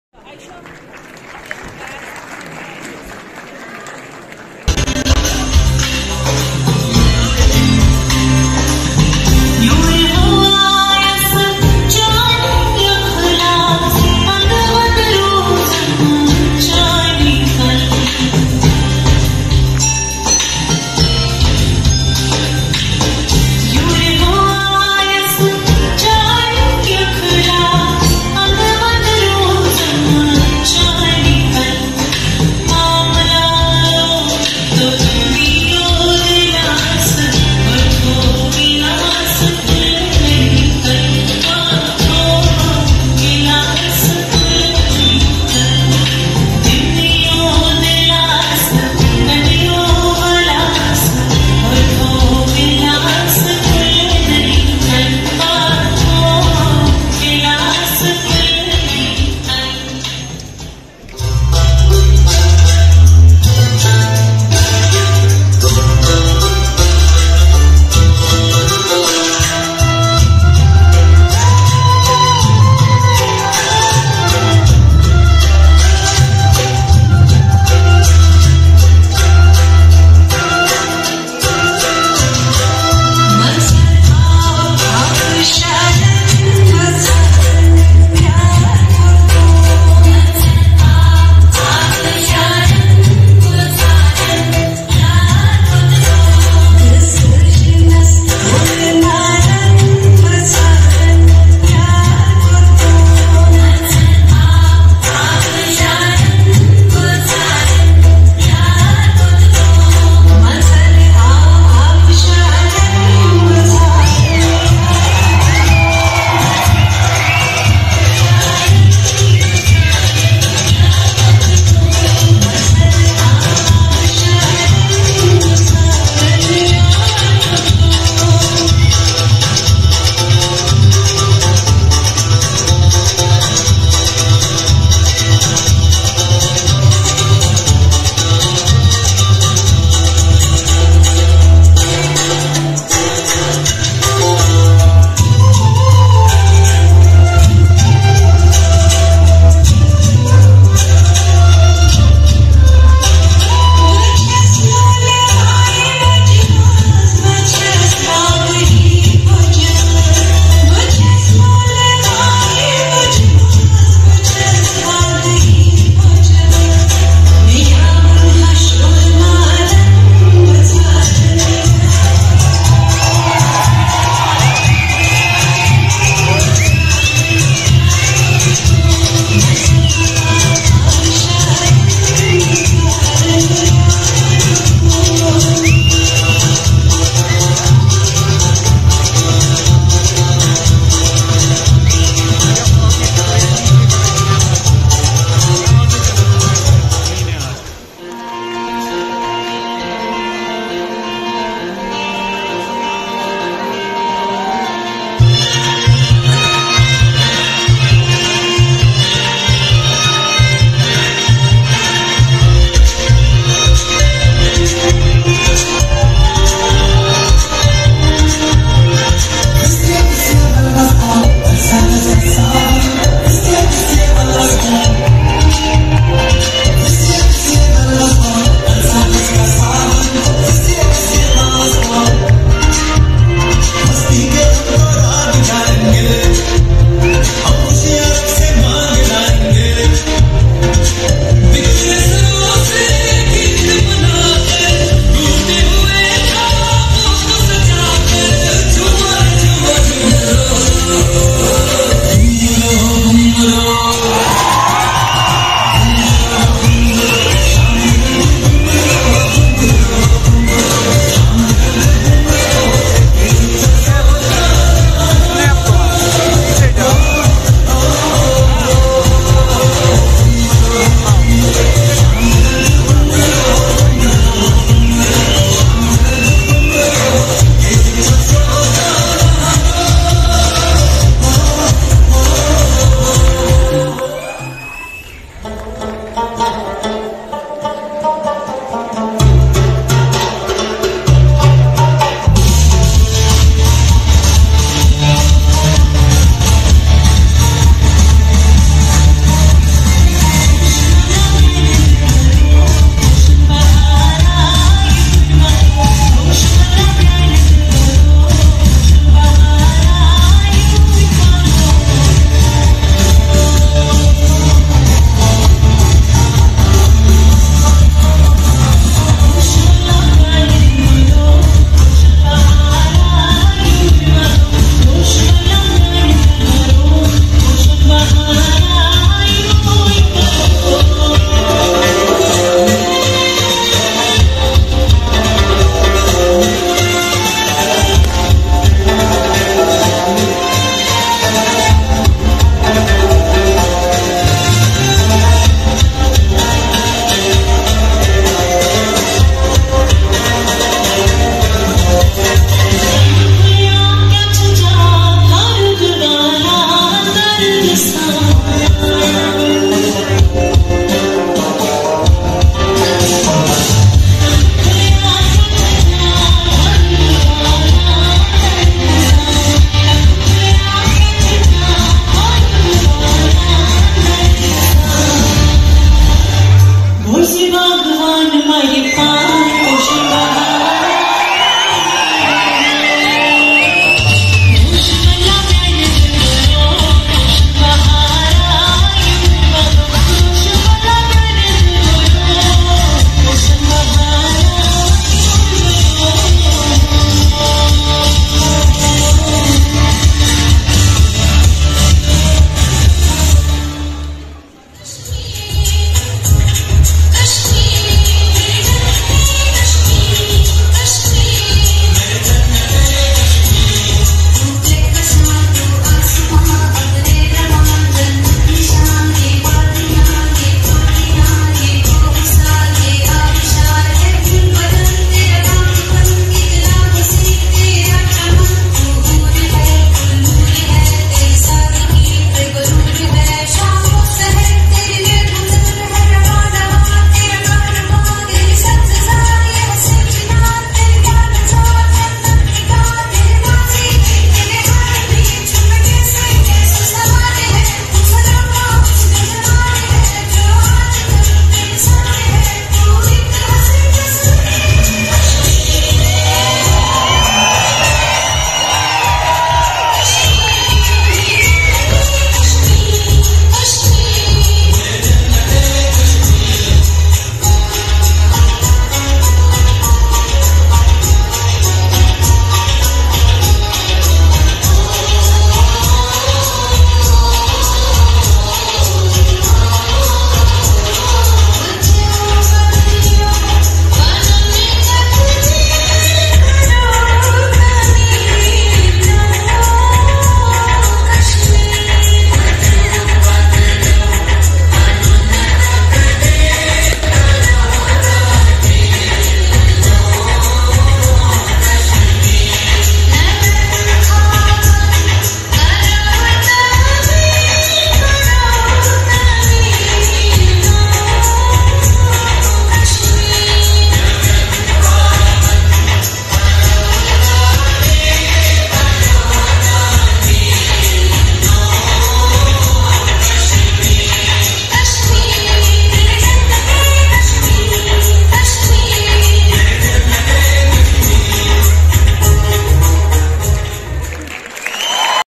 popular Kashmiri Rouf Dance at Tech Fest | University of Kashmir Hazratbal | Day 1
University of Kashmir Tech fest2022 , Tech fest 2022 first time in university of Kashmir organized by Institute of technology zakura campus.

Kashmiri songs,